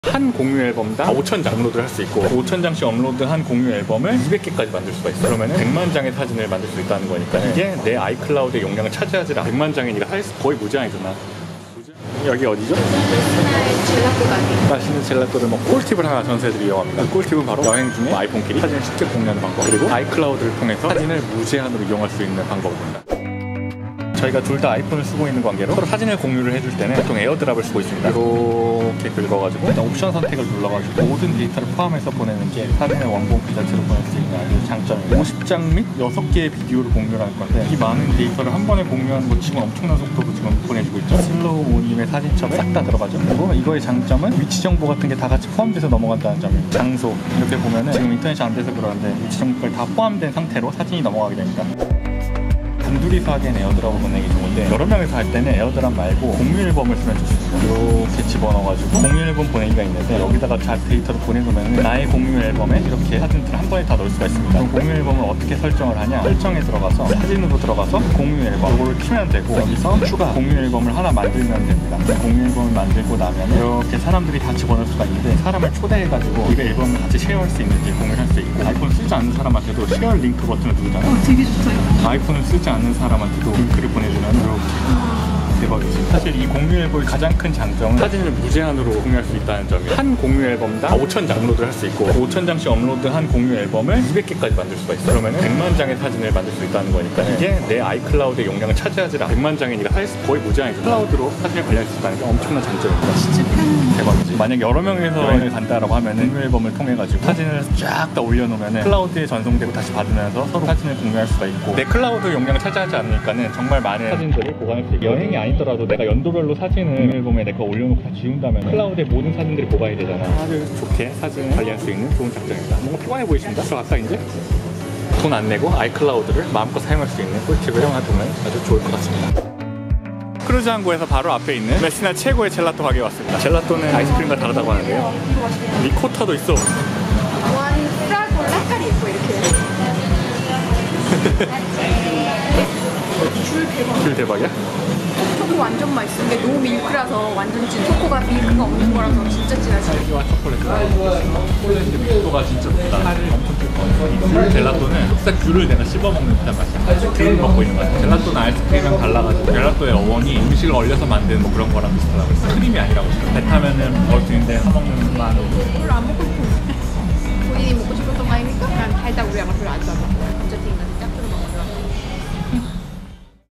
아이폰에서 무료로 아이클라우드에 사진 업로드하는 법 | 공유폴더를 통한 사진 백업하기
00:00 Intro
00:23 에어드랍
00:52 공유앨범
01:26 100만장까지 저장 가능
02:09 번외편 젤라또

안녕하세요
오늘은 예~ 전에 찍어둔 아이클라우드에 공유폴더를 통해서 사진 백업하는 법
그리고 아이폰끼리 에어드랍으로 편하게 사진 공유하는 법을 만들어 보았습니다

아시는 분들은 다 아시겠지만 공유폴더가 내 아이클라우드 용량을 차지하지 않아서
정말 너무 좋은 기능인 것 같습니다.

무제한은 아니지만 100만장이 사실상 거의 무제한 급으로 업로드가 가능하니
많이 이용하시면 좋을 것 같습니다~~

특히 여행중에 공유앨범을 사용하면 친구들이랑도 편하게 사진을 공유할 수 있으니
정말 좋은 기능이라고 생각됩니다~~